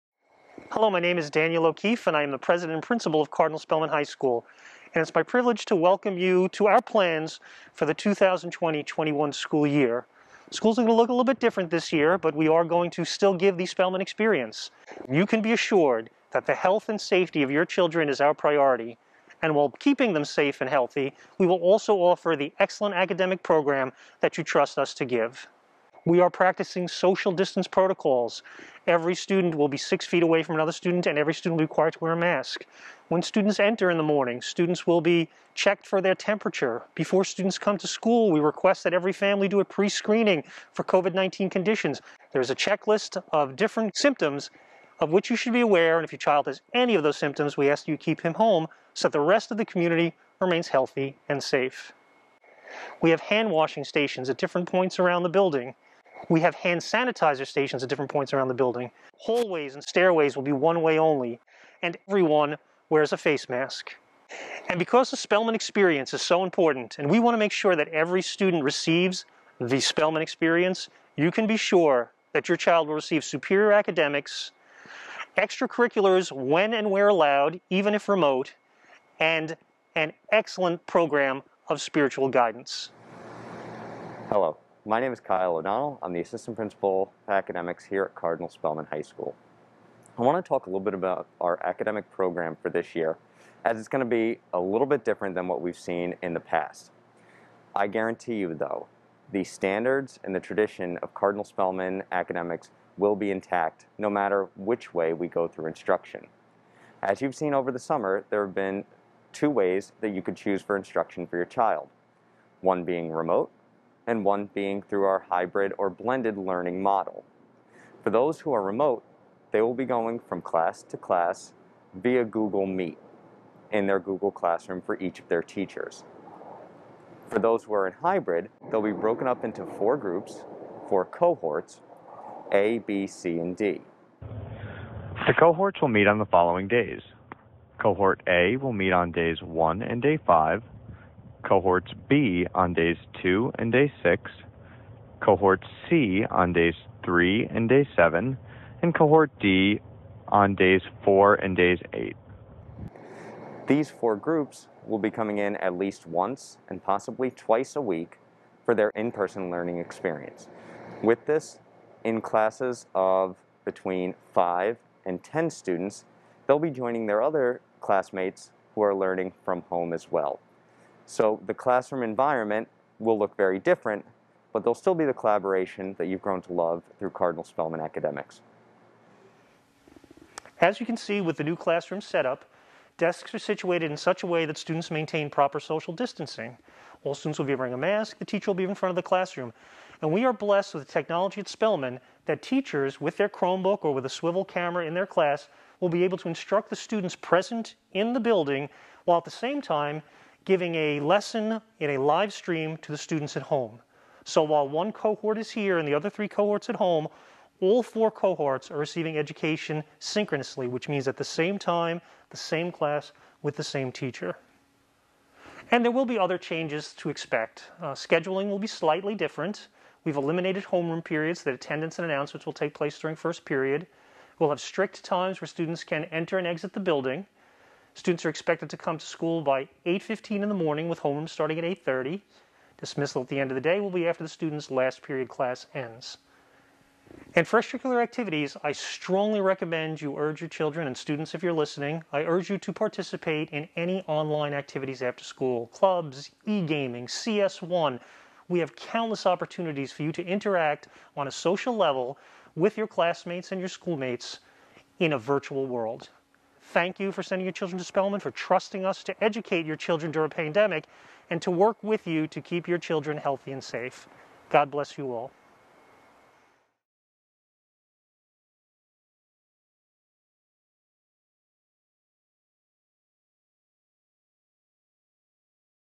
Family Welcome Video
President and Principal, Daniel O'Keefe, and Assistant Principal of Academics, Kyle O'Donnell welcome our Pilot Parents and Guardians to the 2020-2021 school year with a look at some of the changes we have made to ensure the safety and full Spellman experience of your child.